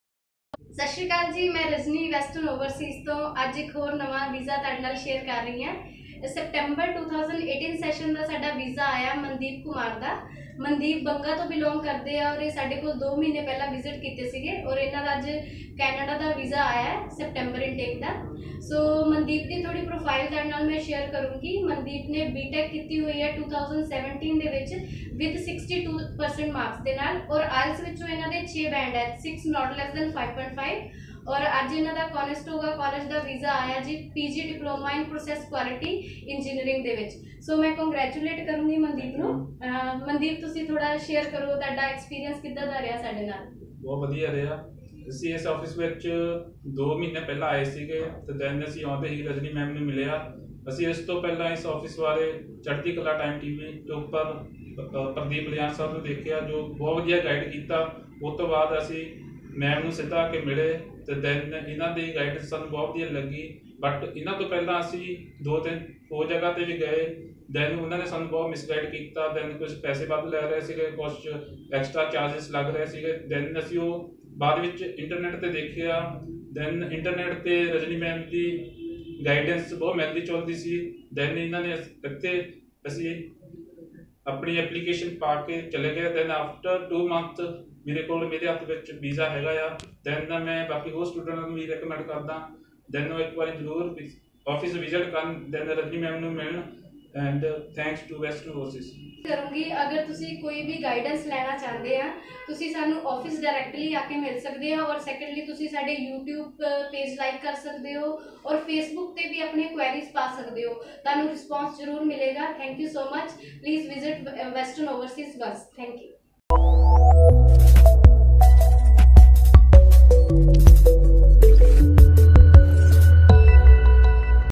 Canada Visa | Conestoga College | September 2018 Intake
Canada Visa Success for Sept. 2018 INTAKE.
Congrats to Mr. Mandeep Kumar for Canada Visa of Conestoga College